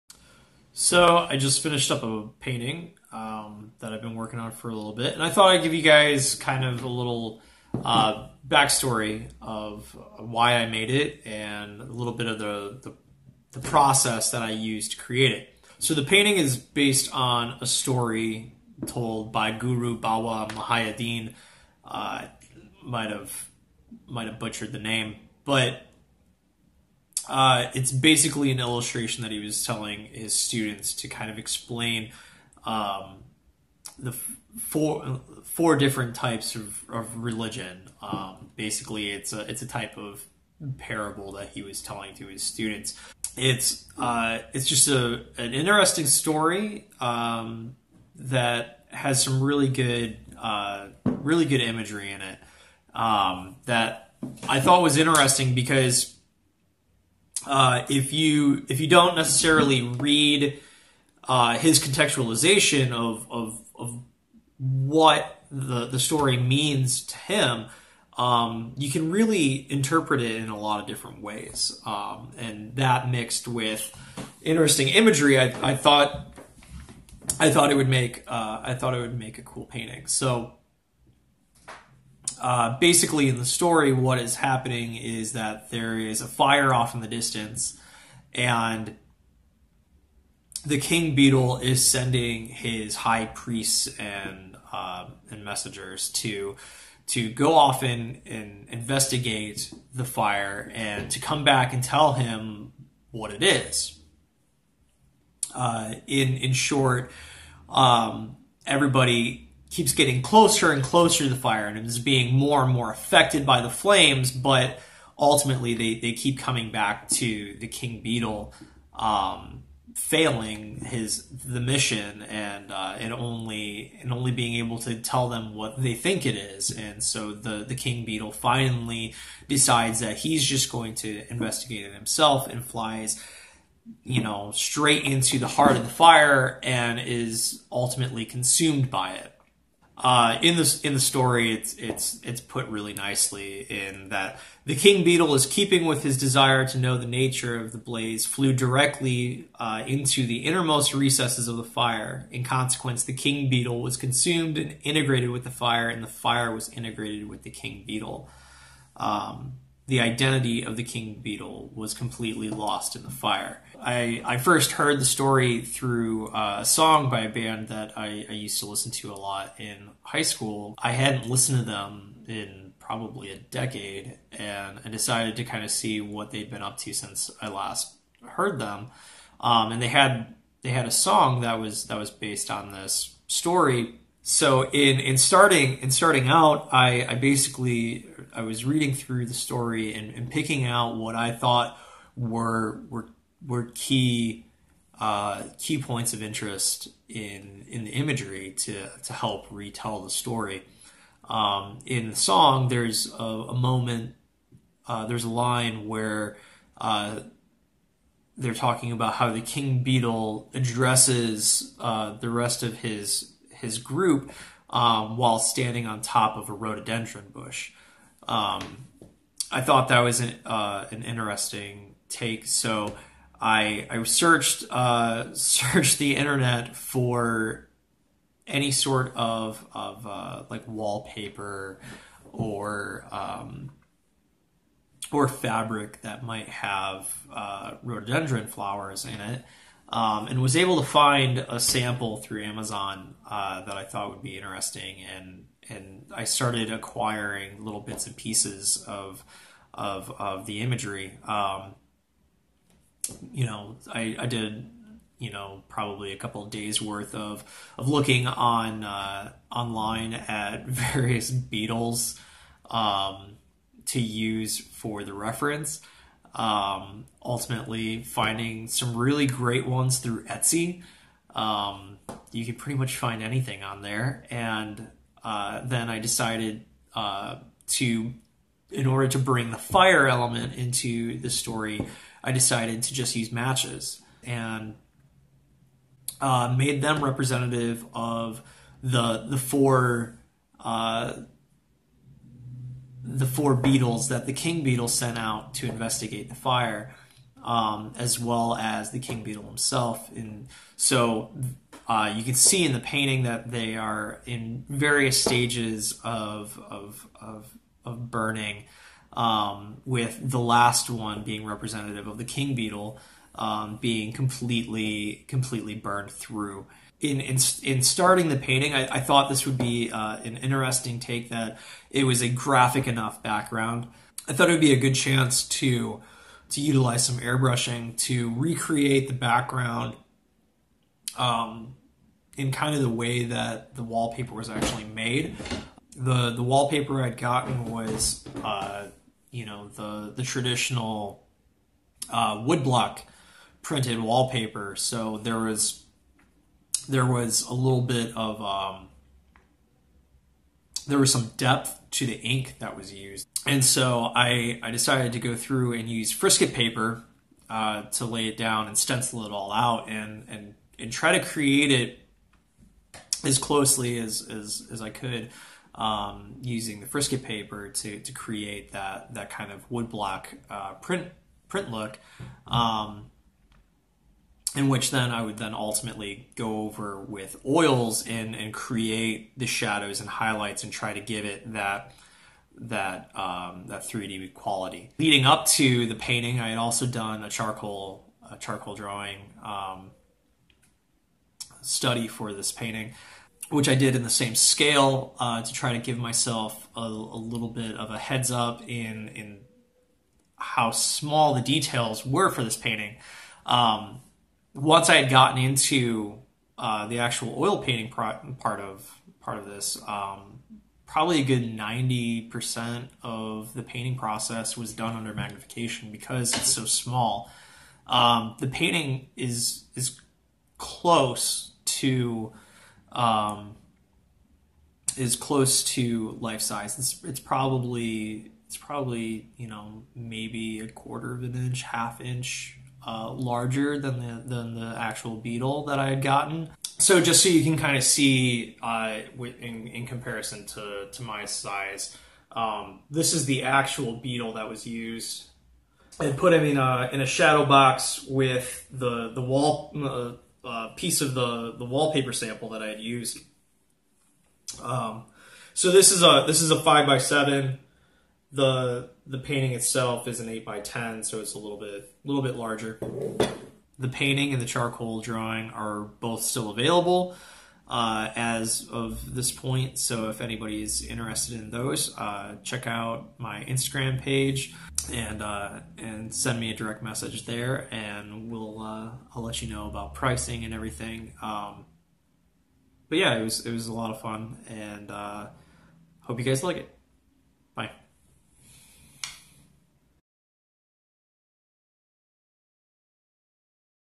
About The Painting: The King Beetle
I share the story behind my painting "The King Beetle"